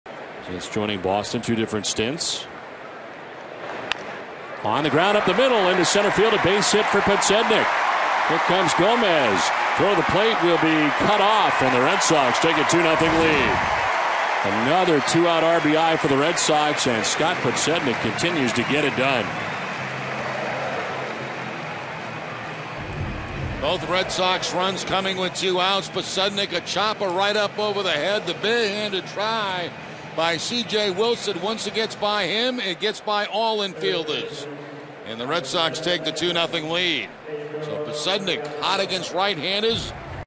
2012/08/23 Podsednik's RBI single
LAA@BOS: Podsednik's single gives Red Sox 2-0 lead